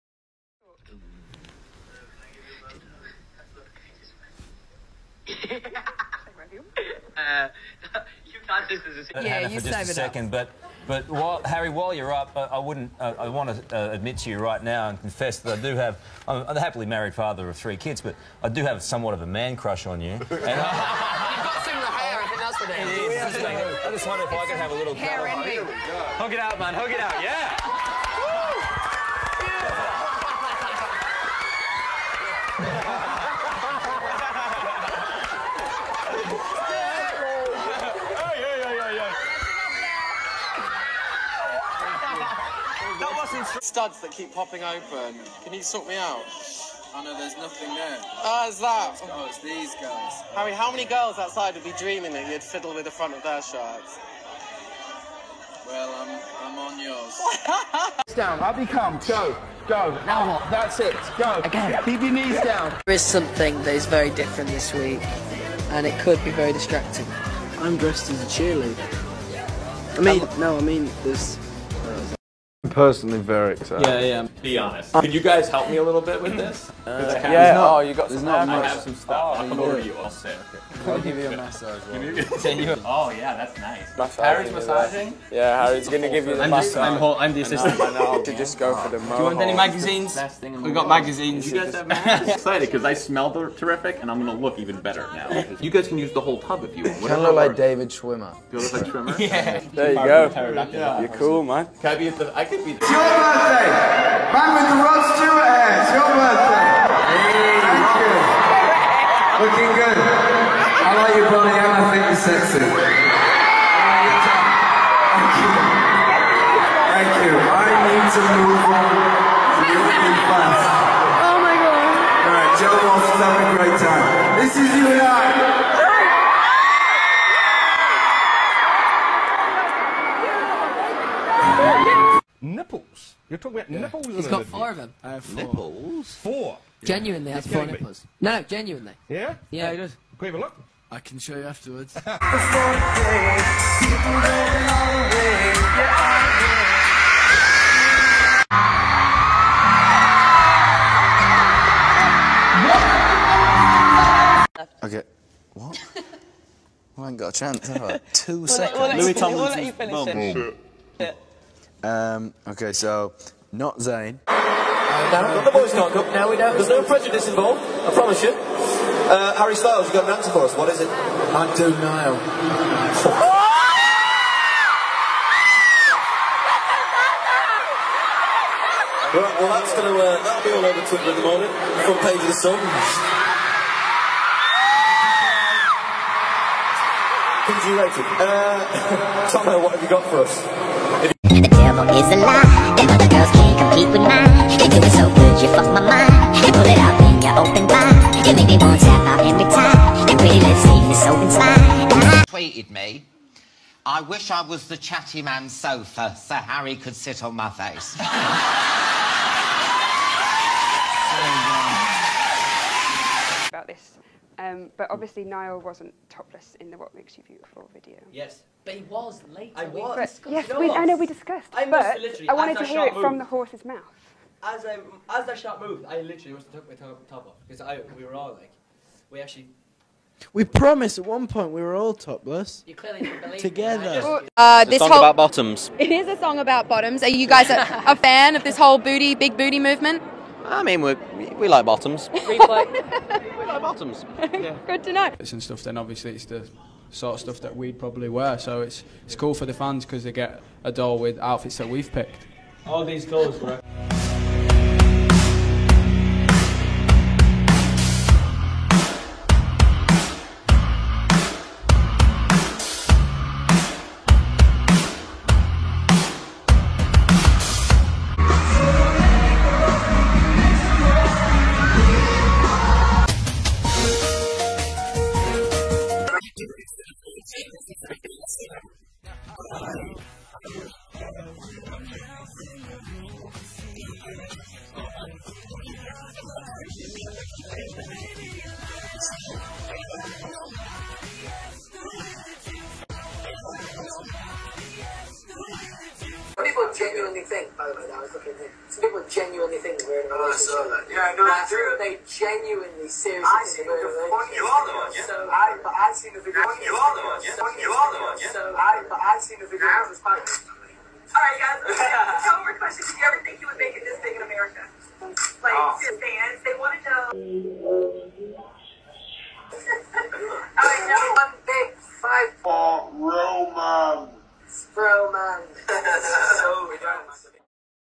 Harry Styles being gay for 5 minutes not so straight
This is not meant to offend anybody in anyway, it is simply just for entertainment purposes and of course I know Harry has decided not to put a label on his sexuality, this is just some videos that I put together in which he is leaning towards being in the closet however none of it should be taken seriously.